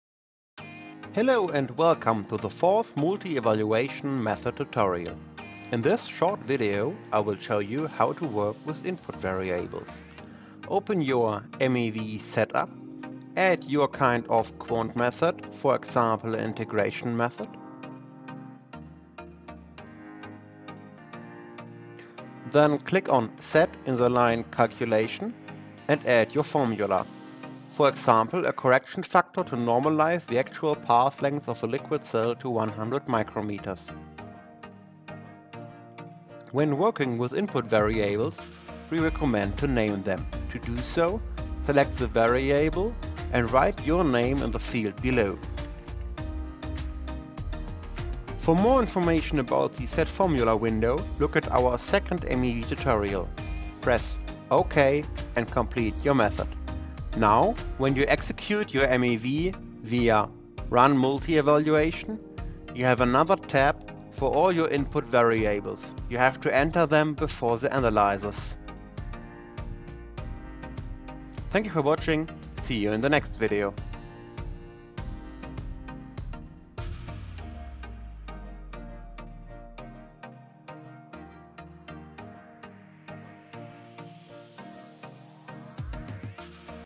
Multi Evaluation (MEV) Workflow 4 of 4 | OPUS Tutorials | FT-IR and Raman
In this 4 part tutorial, we introduce you to setup, management, and use of the Multi-Evaluation Workflow.